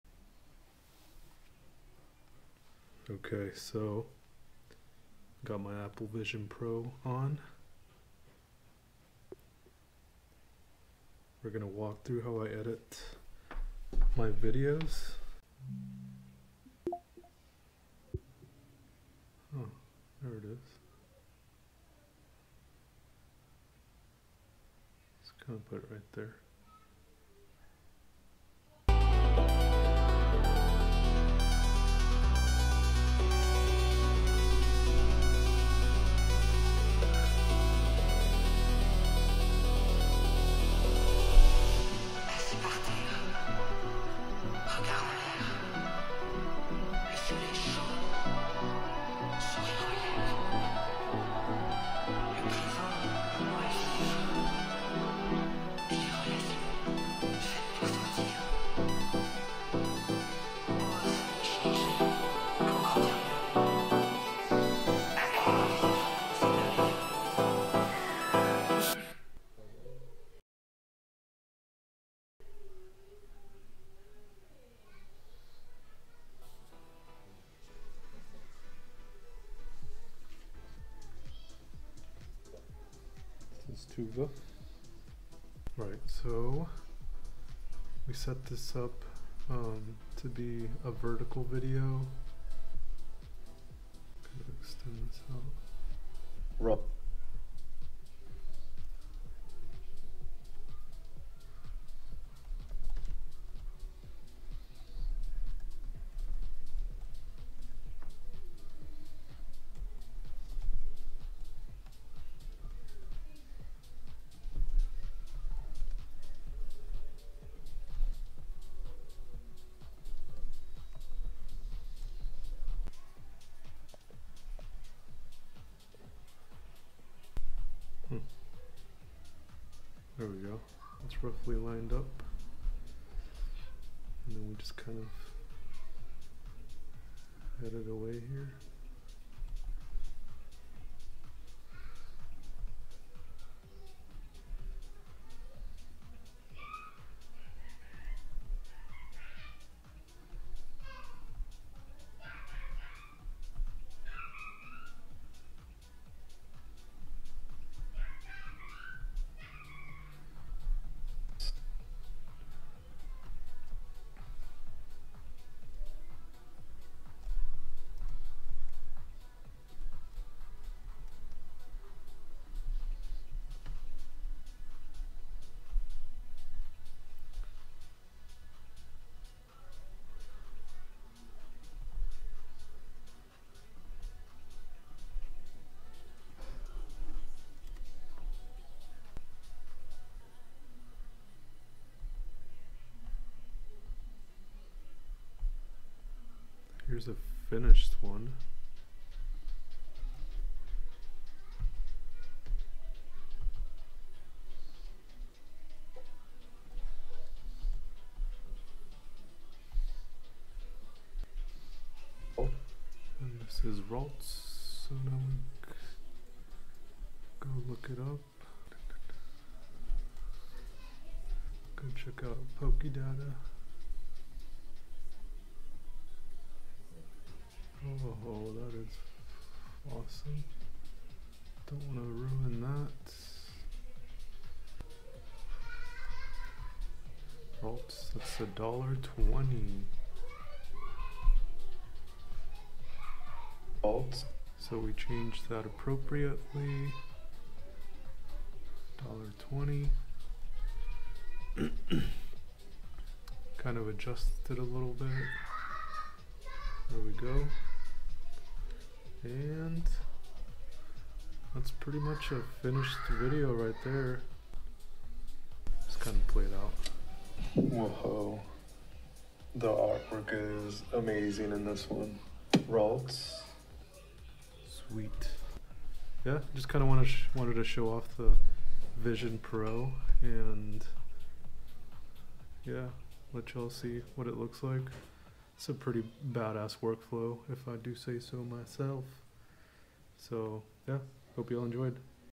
How I Edit Pokémon Videos With The Apple Vision Pro
I show how I edit my videos in Final Cut Pro with the Apple Vision Pro!